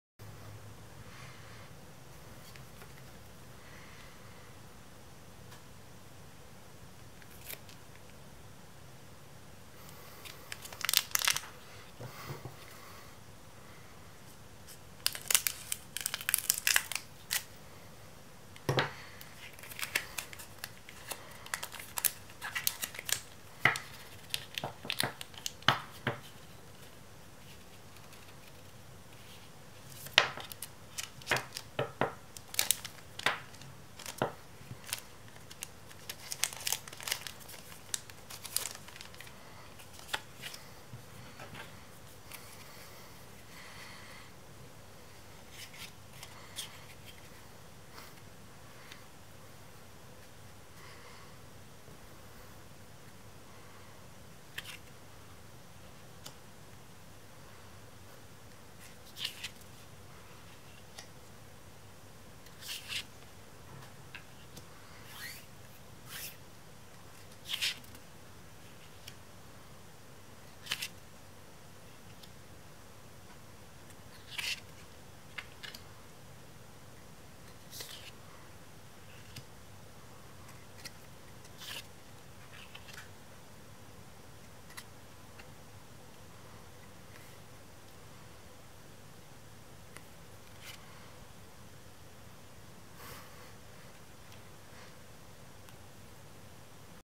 850 points avec un Miasmax 😅 - Unpack Battle
[FR]
Epee et Bouclier - Voltage Eclatant
Ouvrez votre propre booster de la même extension en vidéo et comptez les points !
Qui fera le meilleur score ?

[EN]
Sword and Shield - Vivid Voltage
Record your own unpacking of booster from the same series and count the points.
Who will get the best score ?